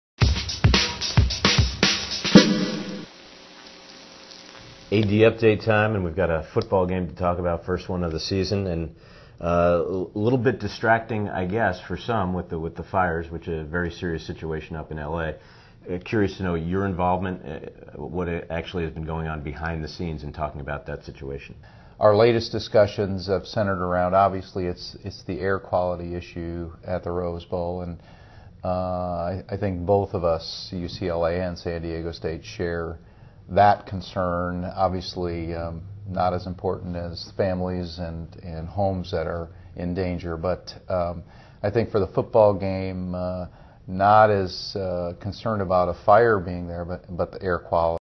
San Diego State Aztecs Football
A dicussion on the wild fires in Southern California and their potential impact on the Aztecs' upcoming game.